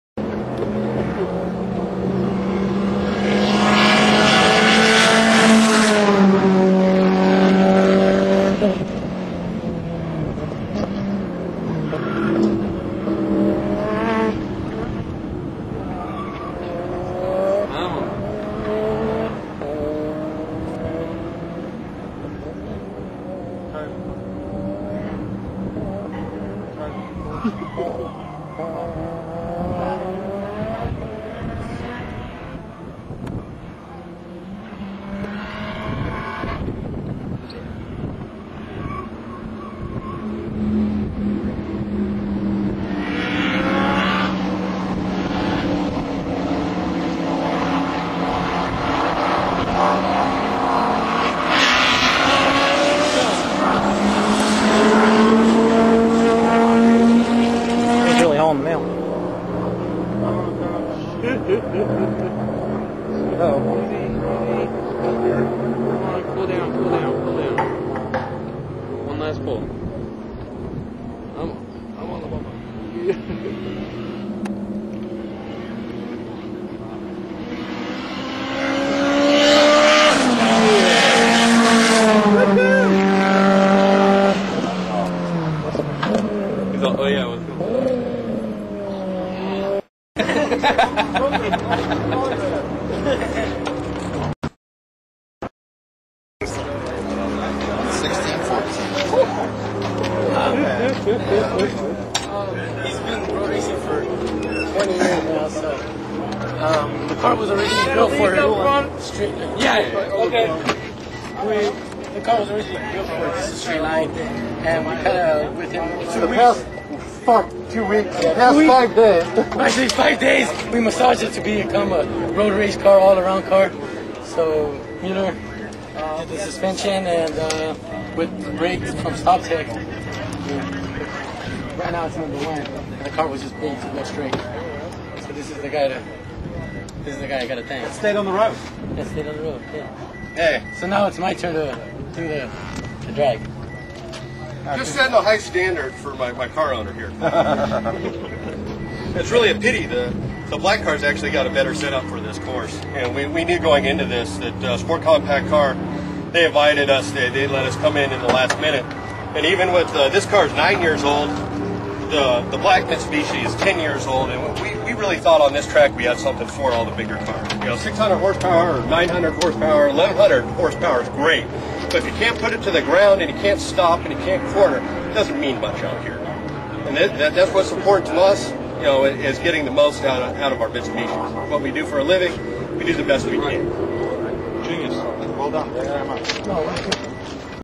Royz 2G GSX - Ultimate Street Car Challenge
Driven by John Mueller in the road course competition part of the SCC Magazine Ultimate Street Car Challenge